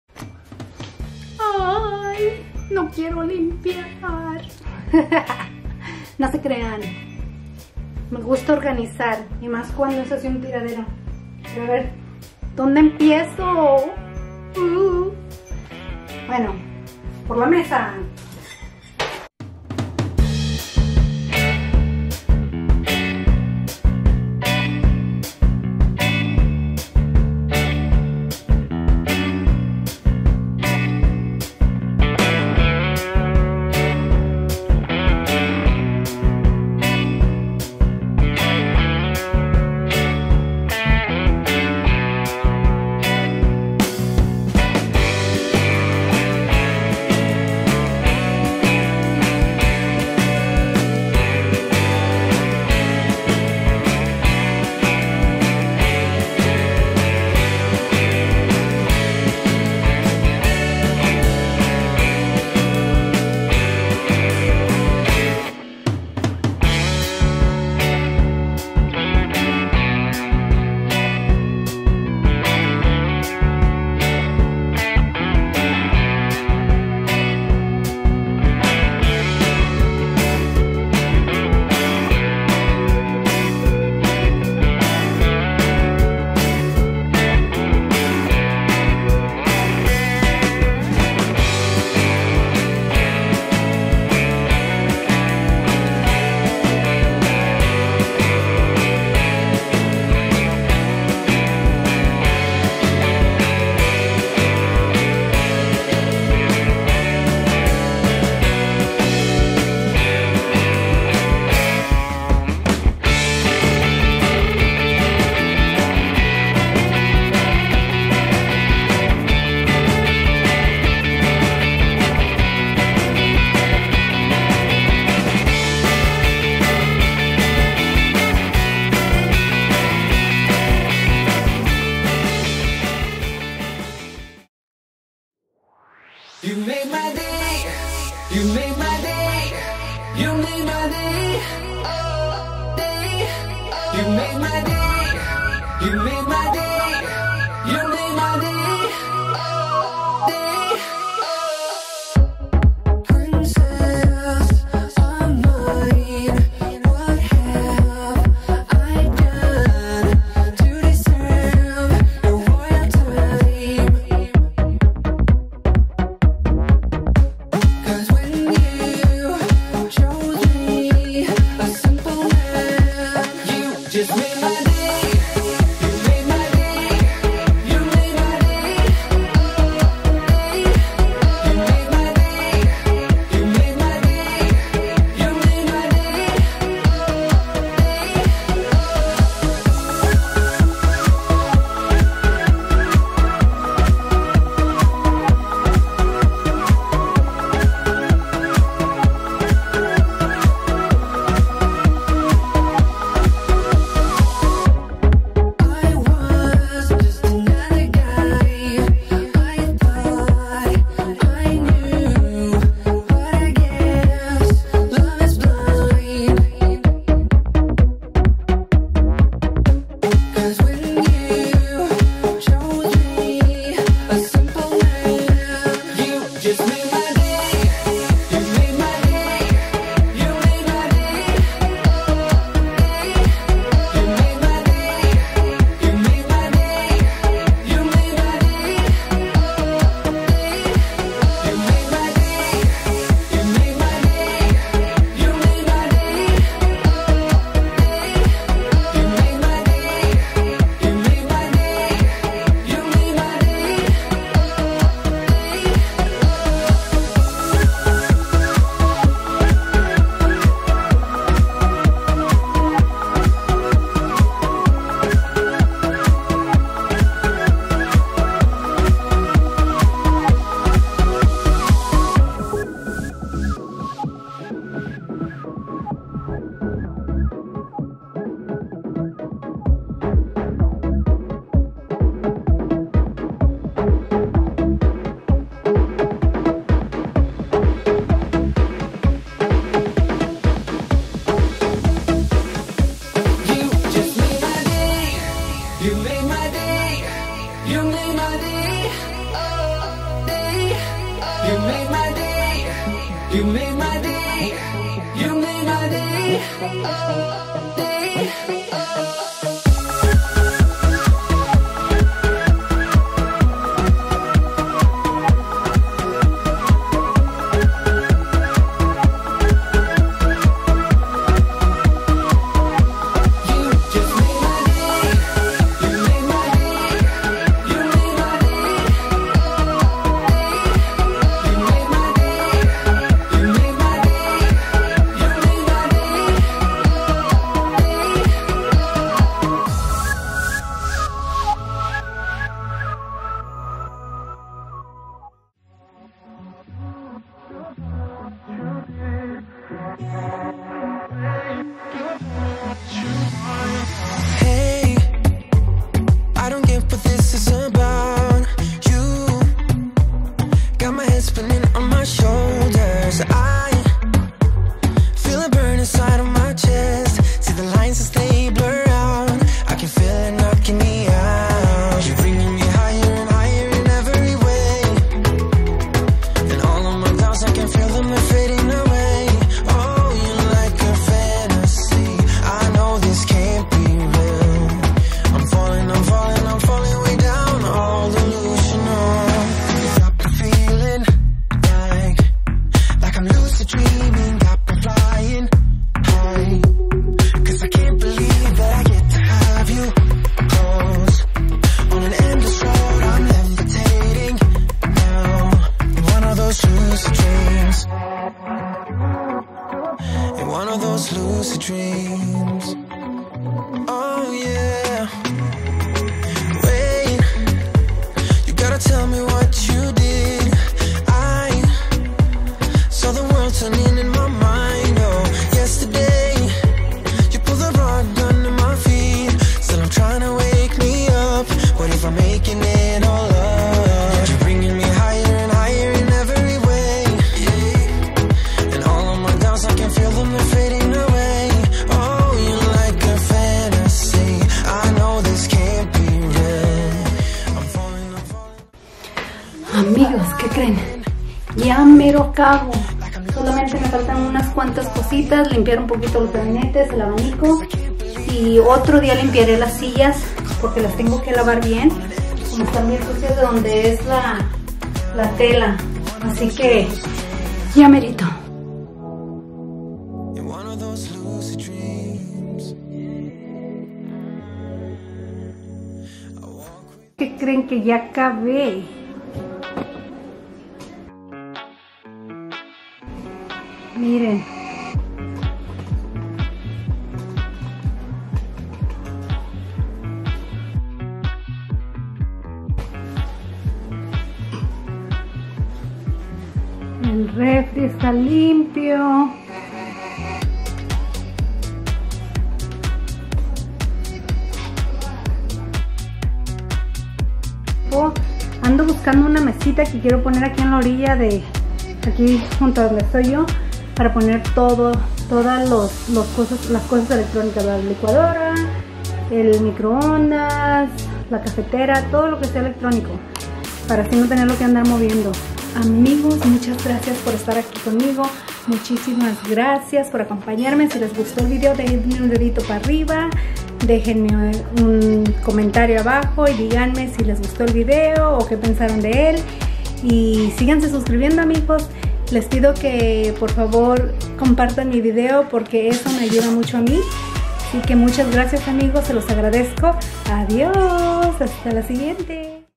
Limpieza De Primavera #2 ( Marzo 2019 ) Organizar/trabajo de casa
Llego la PRIMAVERA!! amigas y amigos hay que limpiar nuestras casitas. A mi me encanta organizar desastres. Si ya sé no es normal. Mi esposo no lo entiende. Pero a mí me satisface hacerlo. Vamos amigos limpiemos juntos.